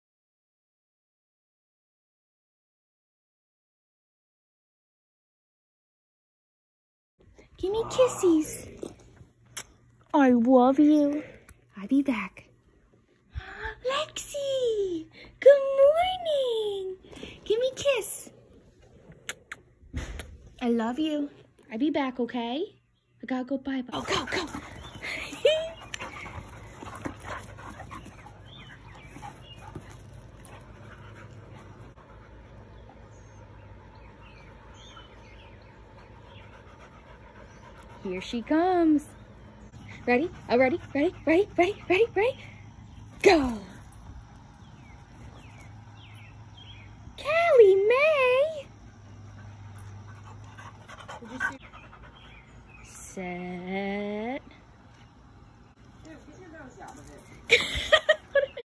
Rough housing 😆 - Day 271
Lexie and Callie have so much fun chasing each other and playing together in the yard!